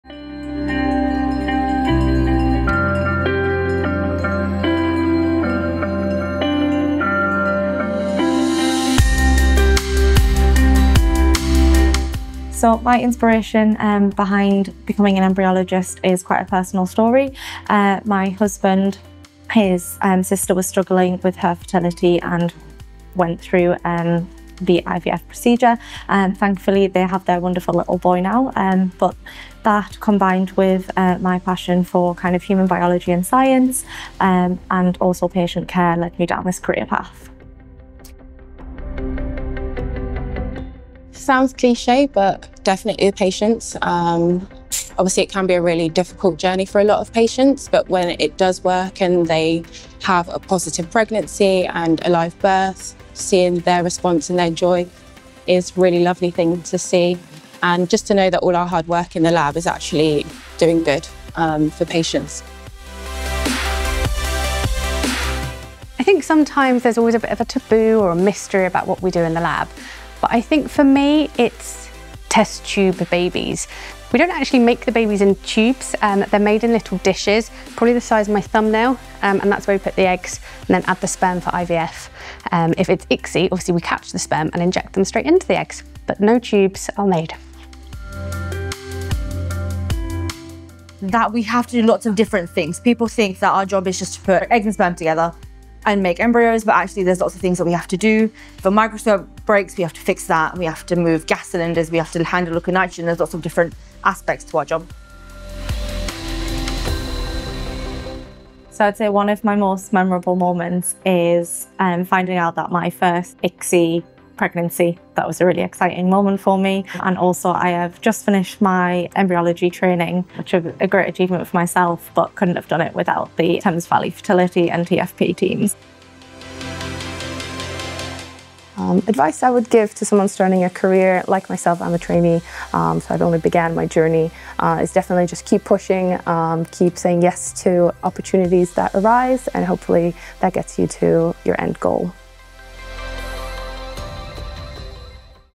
Why I Became an Embryologist | World Embryologist Day
A personal story from our embryologists at TFP Fertility. ❤️ Discover the passion, science, and care that inspires the journey of helping families grow.

To learn more about our supportive approach, visit us at TFP Fertility.